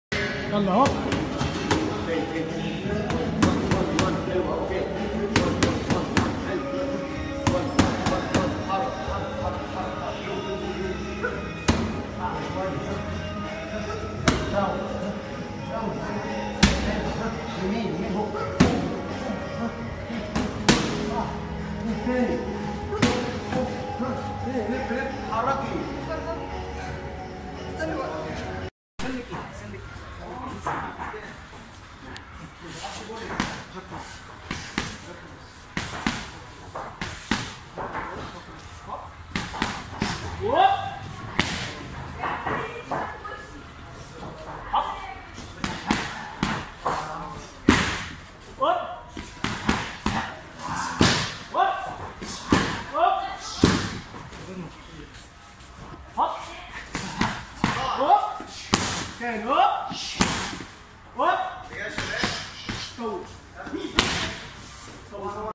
Kempo Techniques Explained: The Art of the Efficient Punch
kempo skills
coach Mostafa Hosny
HanshiIhab Eyada